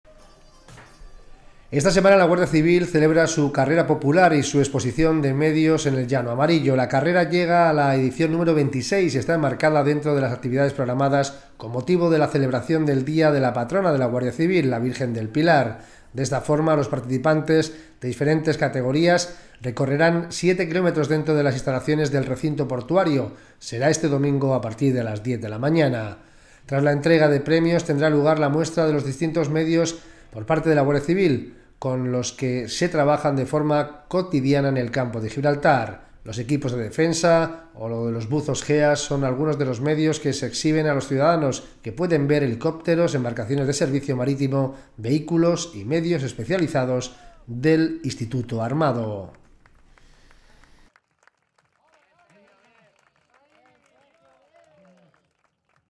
Este domingo se celebra la XXVI Carrera 'Virgen del Pilar'
La Guardia Civil celebra su Carrera Popular y su exposición de medios en el Llano Amarillo. Será la vigésimo sexta edición de una prueba popular, en la que todos aquellos que quieran participar tienen la opción de inscribirse a través de la página web, Crono Finisher